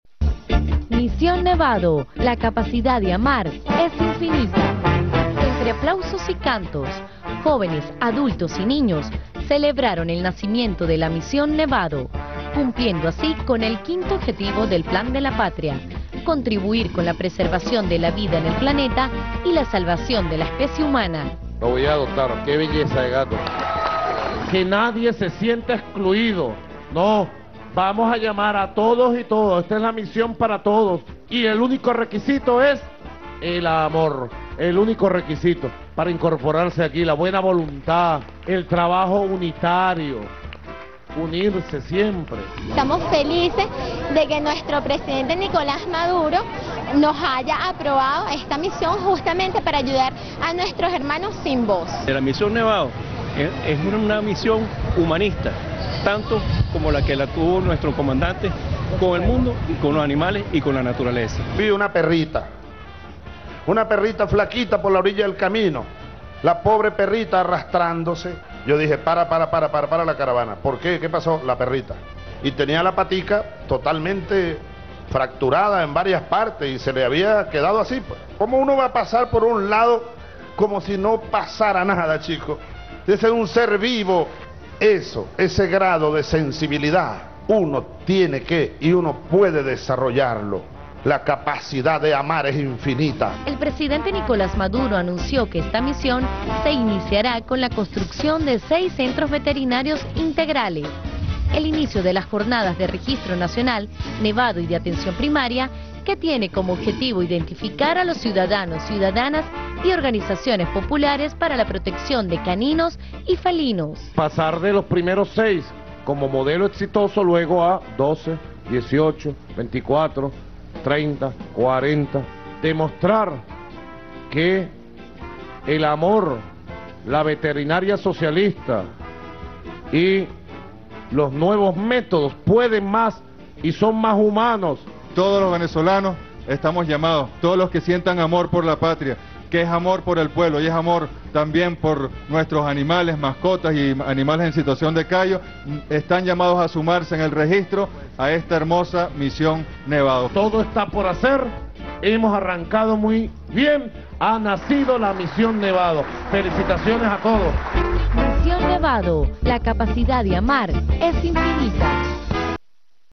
Micro-resumen sobre la actividad de lanzamiento de la Misión Nevado
Realizado por Venezolana de Televisión.